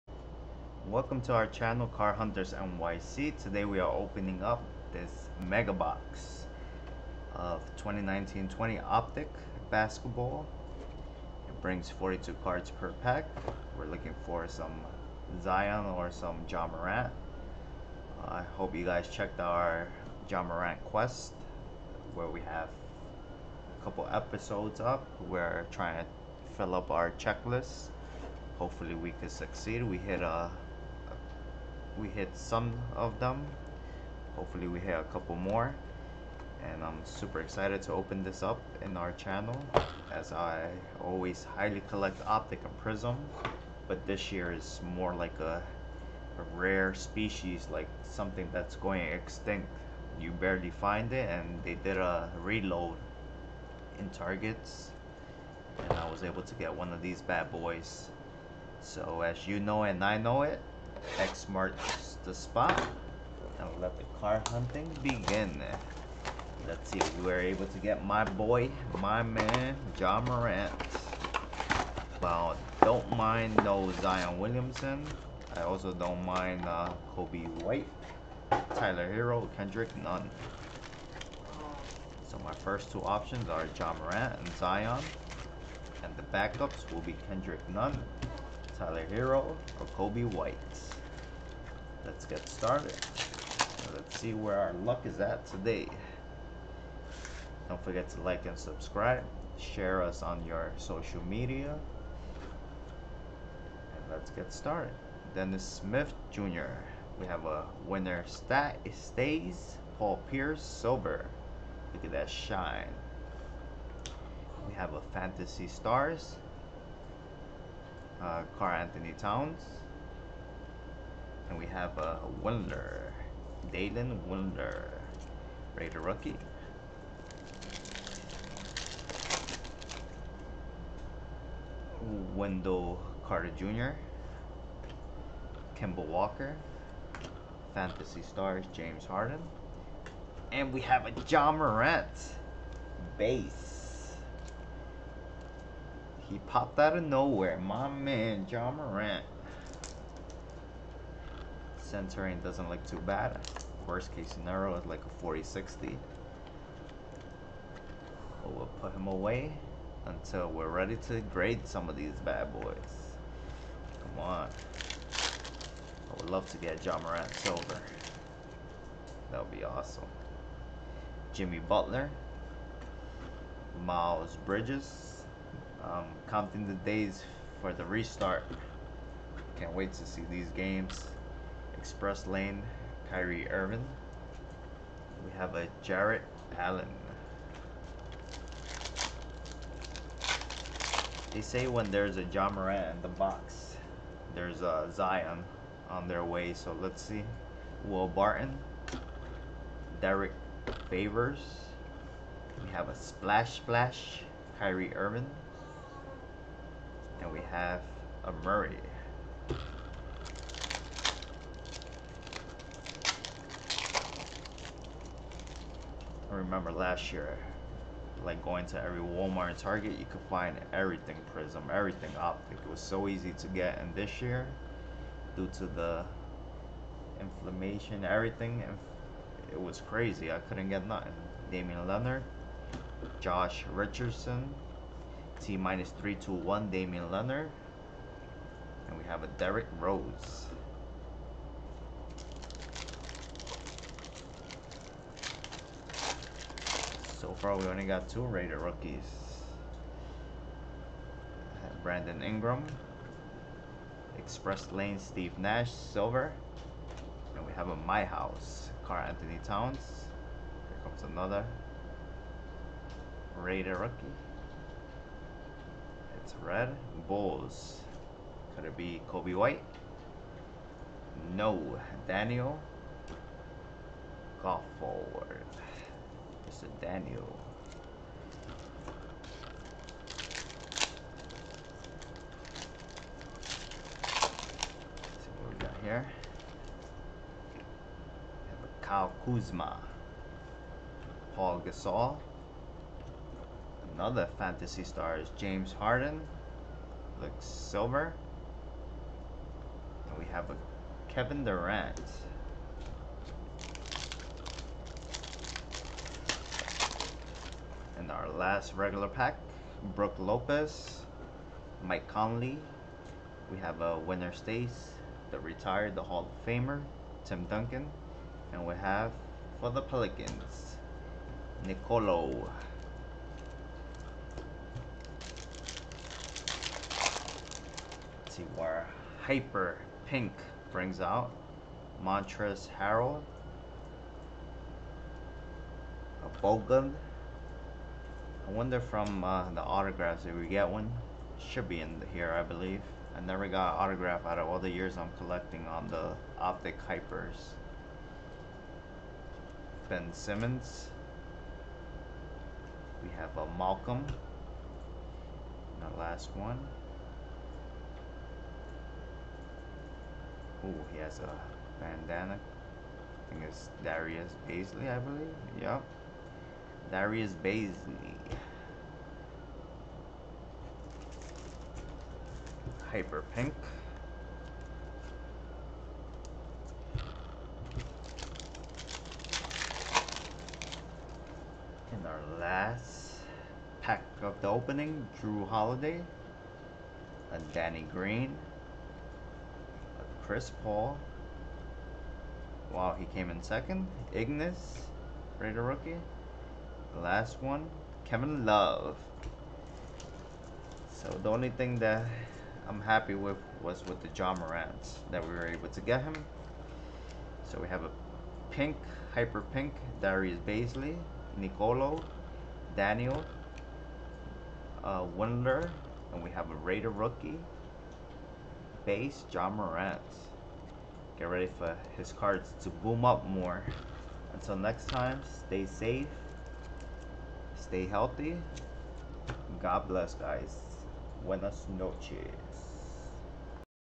Ja Morant Sighting!!! 2019-20 Optic Mega Target Box
was able to grab a box from a third party seller. was more than happy with what i pulled